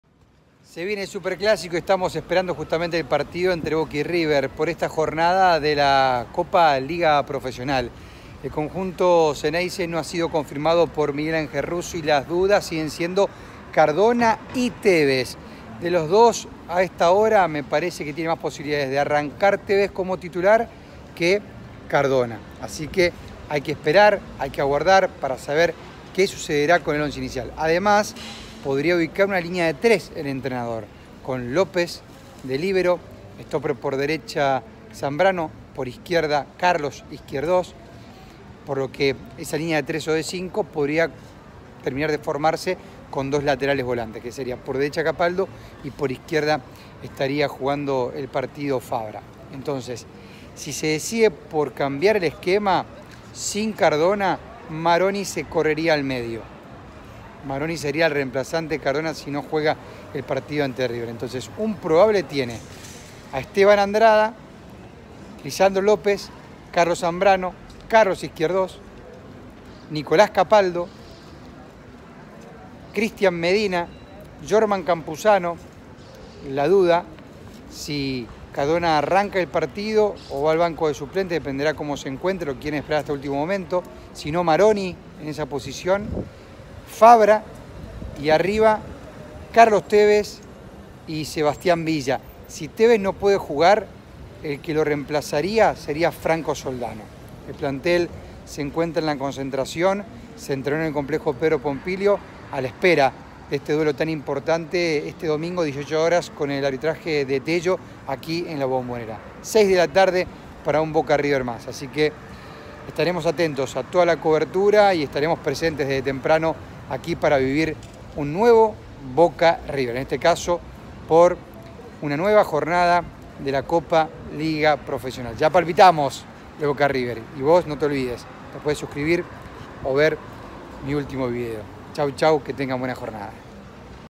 Boca recibirá a River ¿Querés saber cuál será el equipo de Boca Juniors? Dale Play
Se viene el Superclásico y Boca aún no está confirmado. Acá te cuento quiénes serían los titulares ¿llega Tevez? ¿Juega Cardona? ¿Línea de 3 o de 5 en el fondo?.